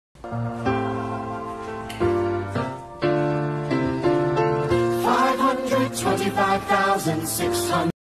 A lot of photos 📸 Credits @haleysvoyage
A youtube short video of a Great Dane meant to brighten your day